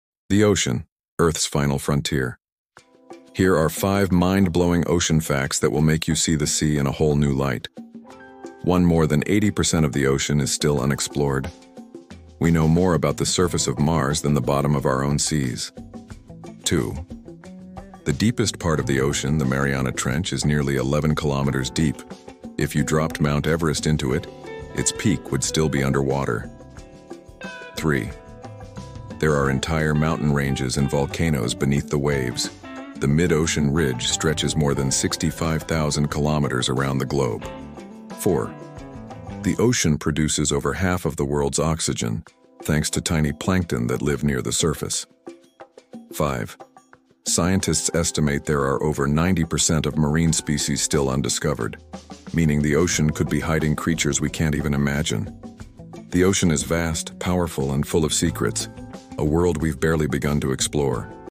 5 Mind-Blowing Ocean Facts You Didn’t Know 🌊
The ocean covers more than 70% of our planet — but we’ve explored less than 5% of it. 🌊 Discover 5 mind-blowing ocean facts that reveal how mysterious and powerful the deep sea really is!